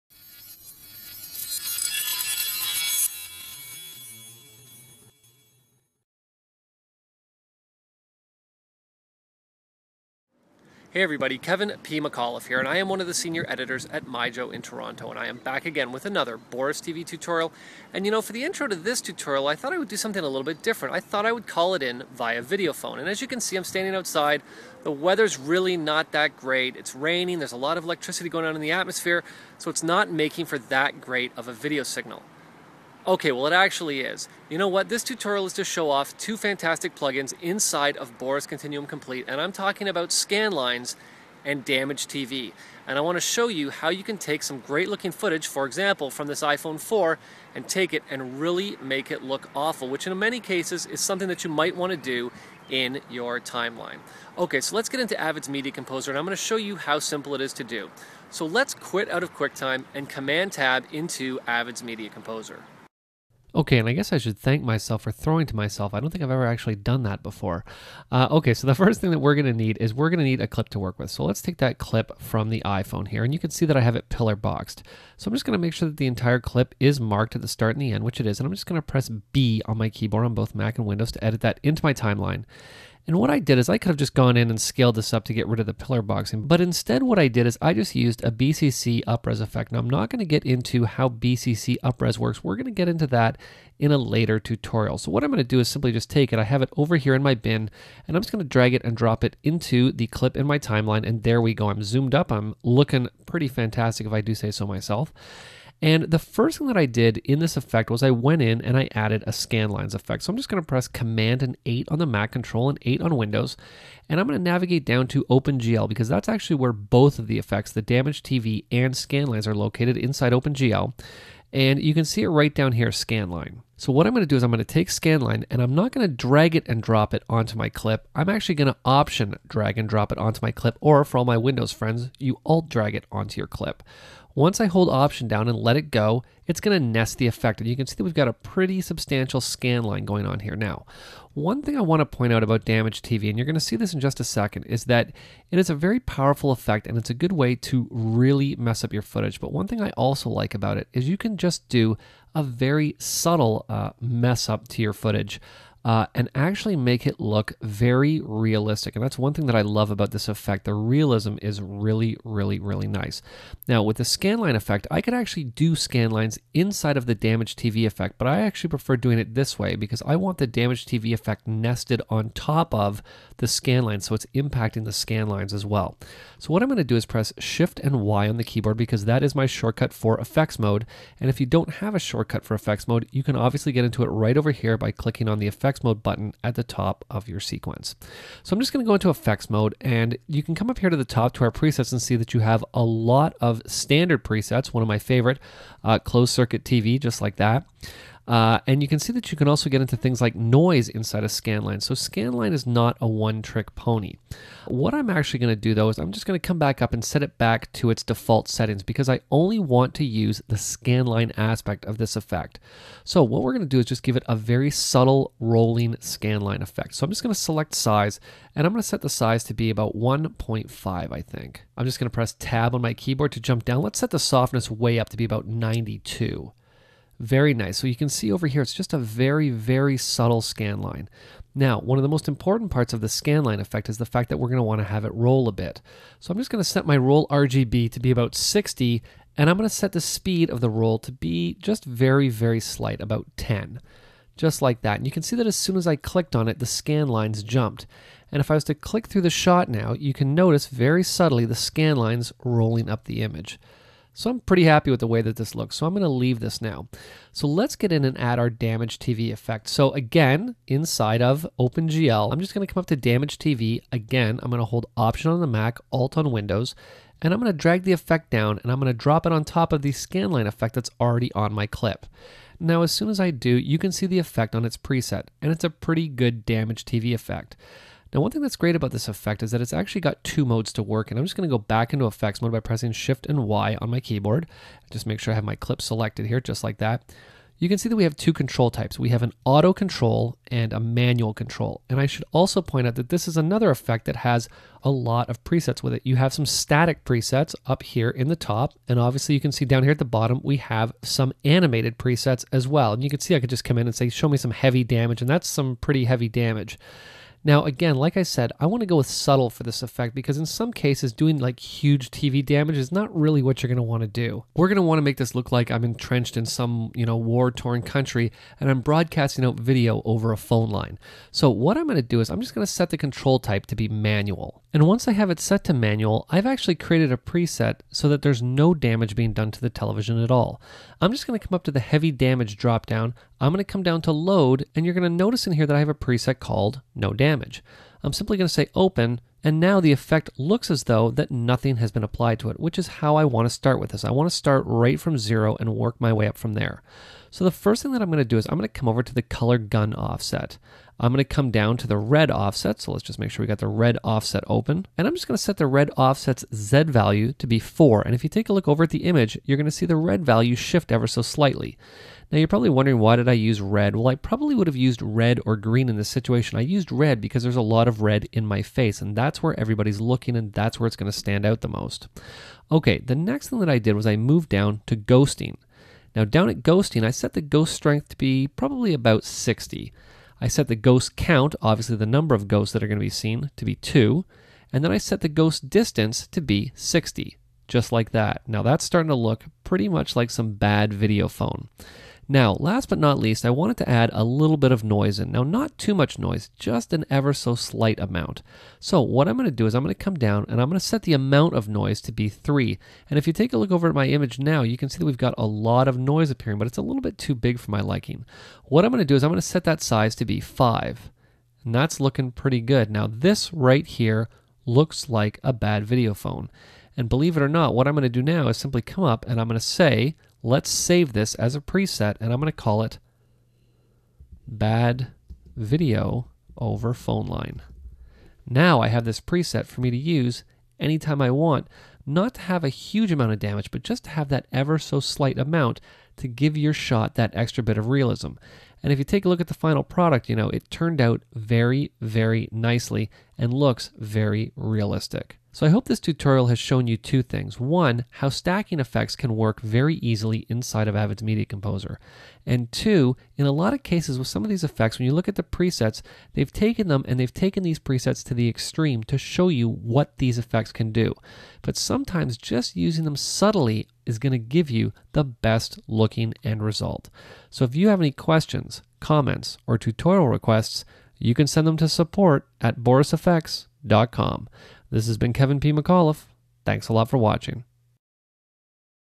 Simulate Cell Phone Video in Avid Media Composer
Simulate the look of low-quality cell phone video with Boris Continuum Complete's Damaged TV and Scanline filters.